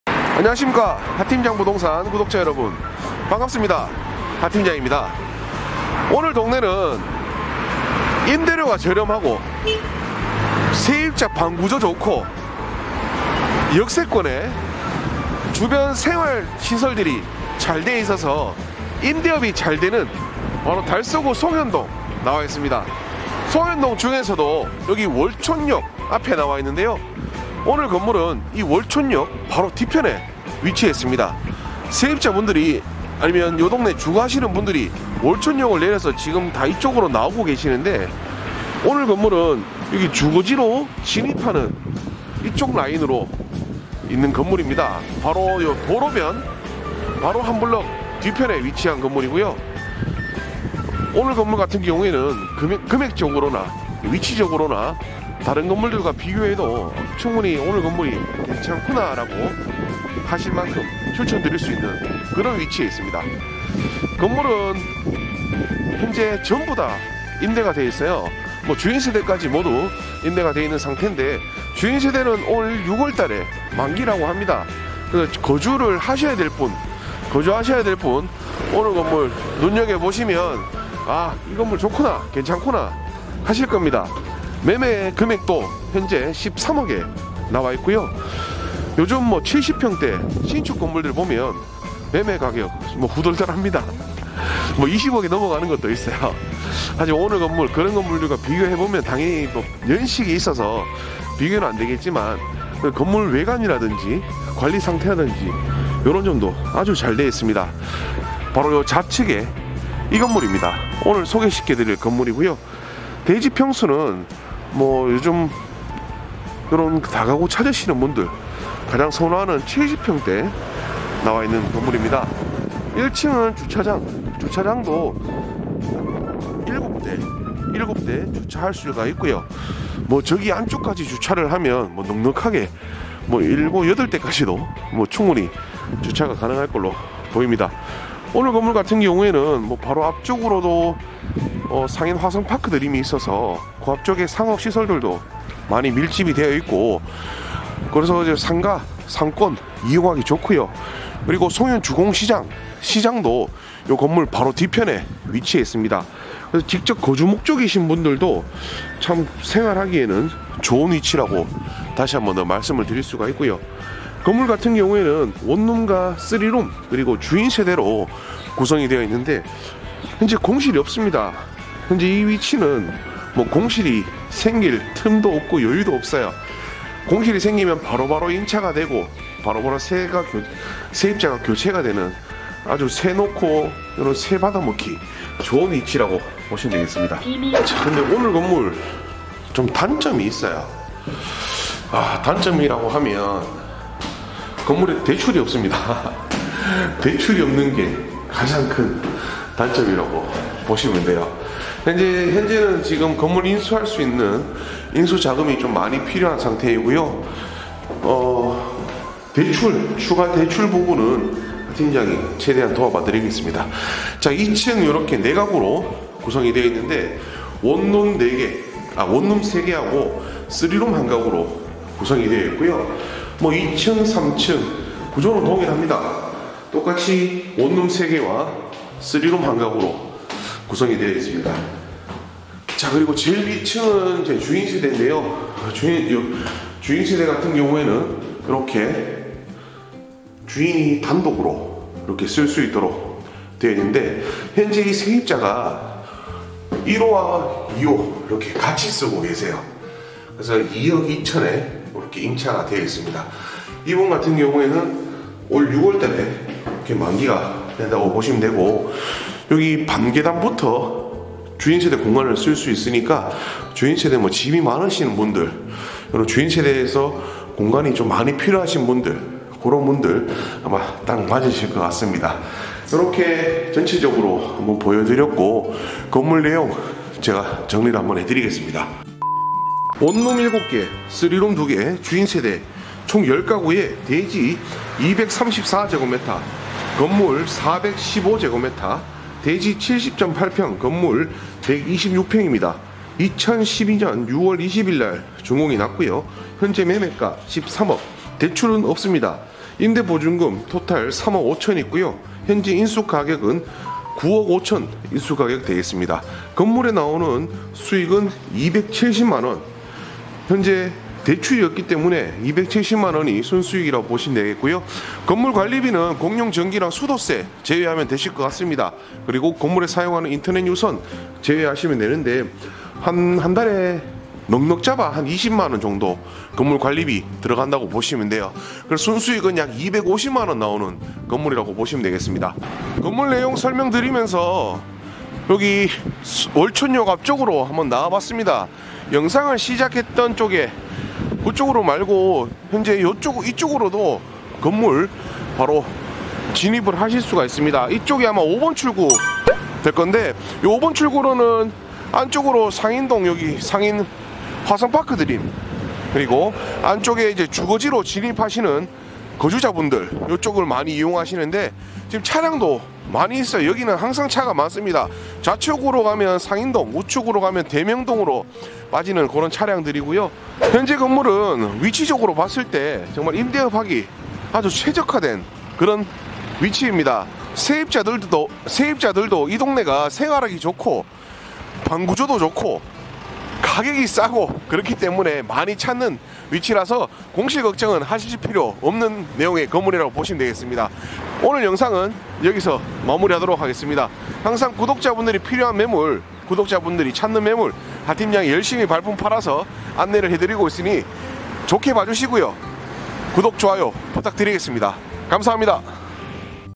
[하팀장]대구원룸매매 역도 가깝고 시장도 가깝고 거주하기 너무 좋은 위치! 세 안나갈 걱정없이 편하게 거주하셔도됩니다^^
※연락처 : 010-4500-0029
※카카오톡 ID : 45000029 (검색)
※해당영상 매물번호:150422

▶소재지 : 대구광역시 달서구 송현동1946-19
▷면적 : 대지 234㎡ / 연면적 415㎡

▶거래형태 : 매매
▷거래예정금액 :13억원
▶중개대상물의 종류: 다가구주택

▷사용승인일 : 2012년06월20일
▶총 층수 : 지하 0층 / 지상 4층
▷주차대수 : 7대

▶방향[주 출입구 기준] :북서
▷입주가능일 : 2022년04월18일이후 협의가능
▶방수[근린생활시설 미포함] / 욕실수 : 방 14개 / 욕실 11개

▷관리비[청소비용,승강기 유지비용 등] : 매월 약 200,000원 예상

◆대표:沈民貞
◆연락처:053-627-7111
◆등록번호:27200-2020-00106